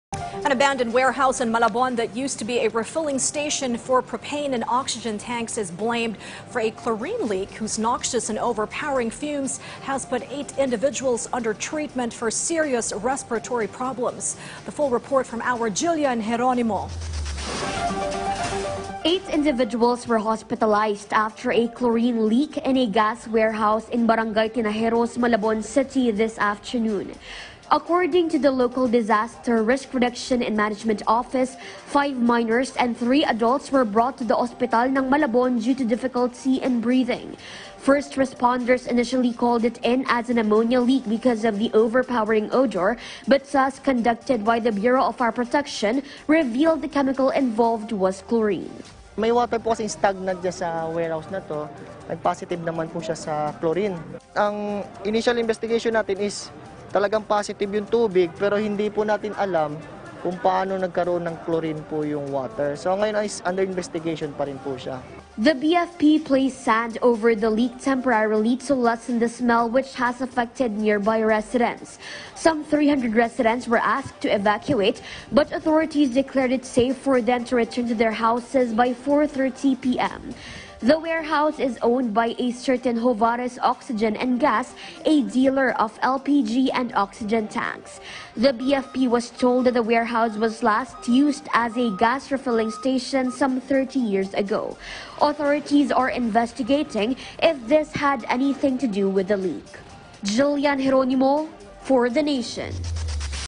8 people hospitalized due to chlorine leak from warehouse in Malabon
8 people hospitalized due to chlorine leak from warehouse in Malabon